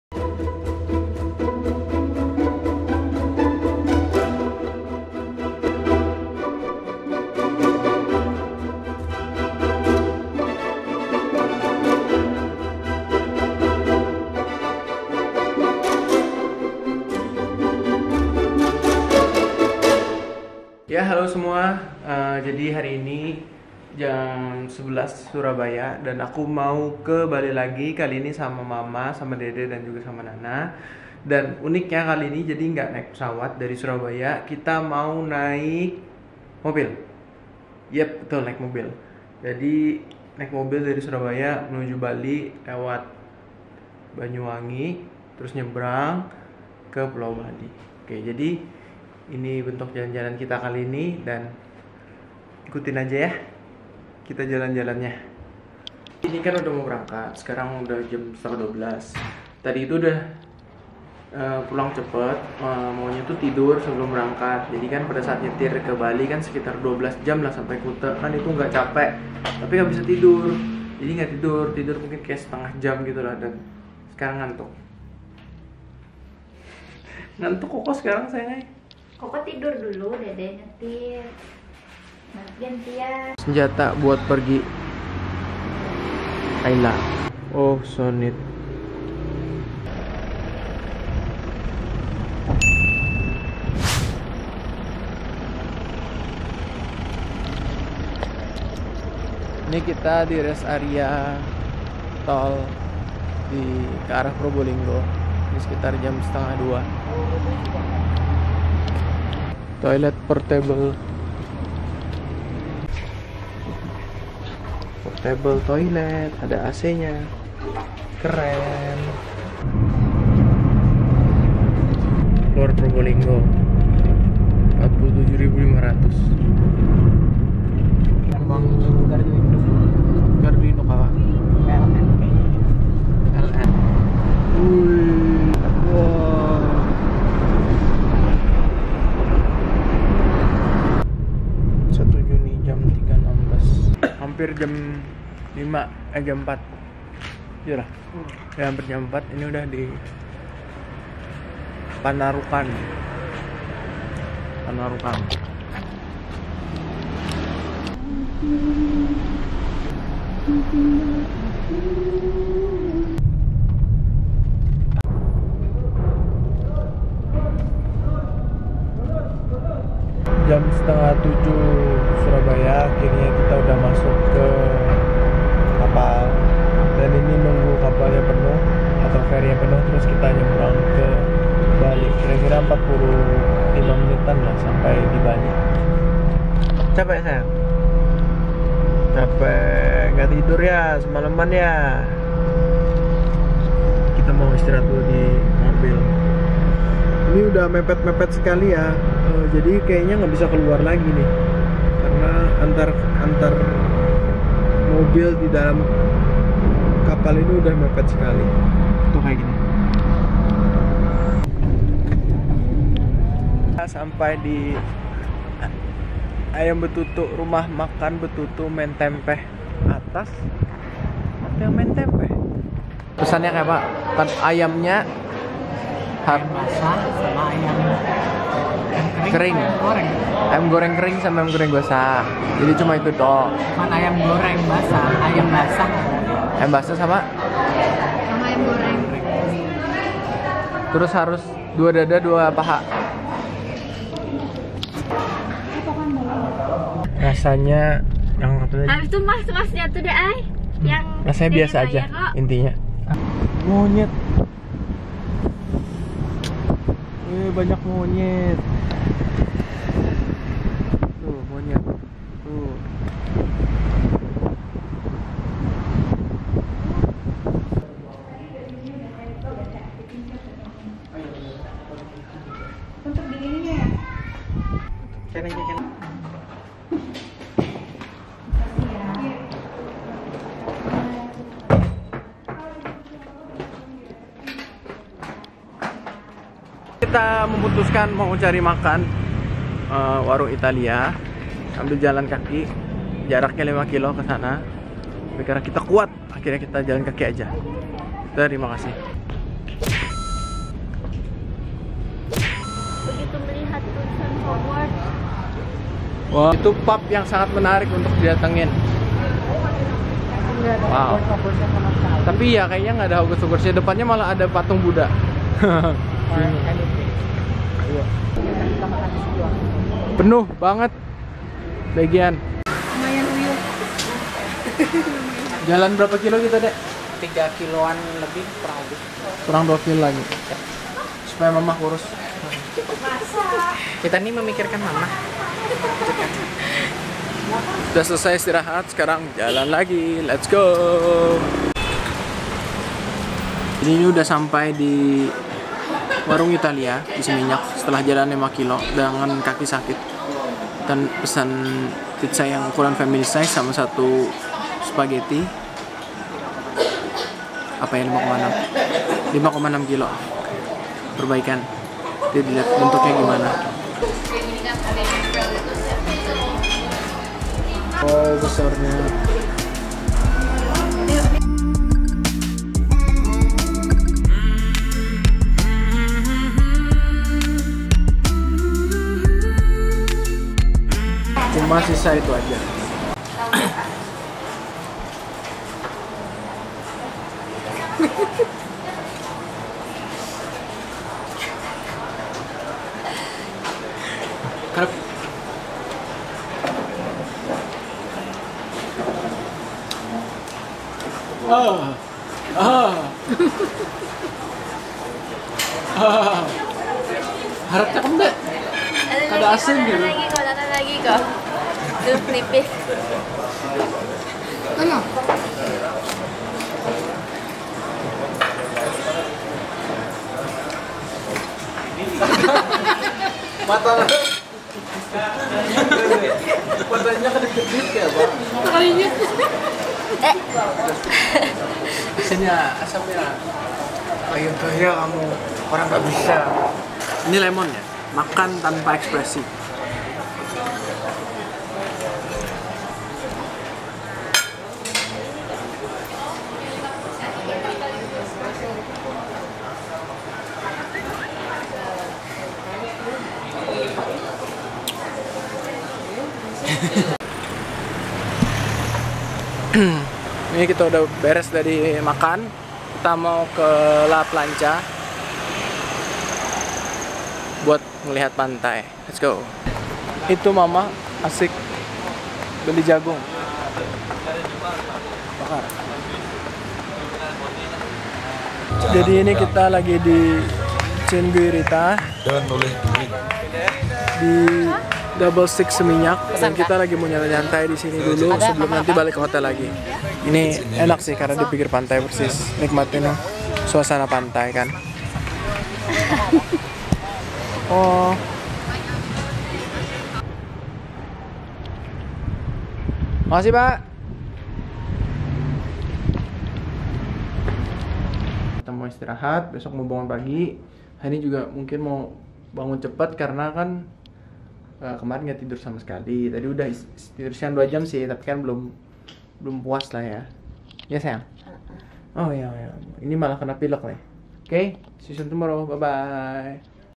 SEKELUARGA ke BALI NAIK MOBIL
capek sih nyetir ke bali dari surabaya naik mobil tapi ada pengalaman tersendiri naik ferry dan ngelewatin jalan naik turun berputar putar sepanjang jawa timur dan bali

semoga menghibur
follow instagram aku di